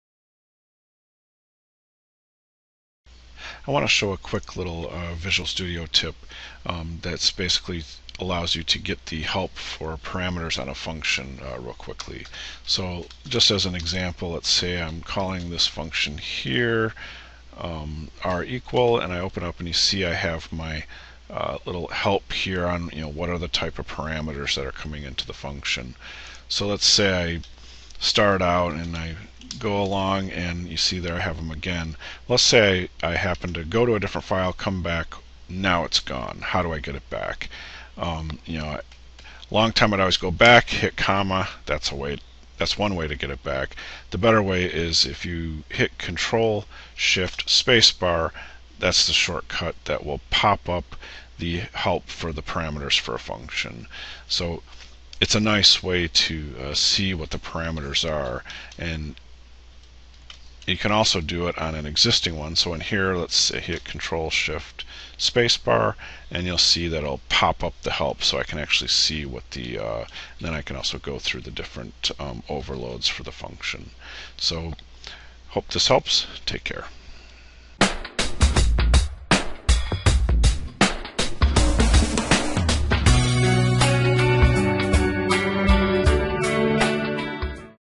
Shortcut to popup function/method parameter help in Visual Studio
Visual Studio tip: if your cursor is located between the parenthesis in a C# function/method you can use use Ctrl + Shift + Space Bar to bring up the function parameter help popup.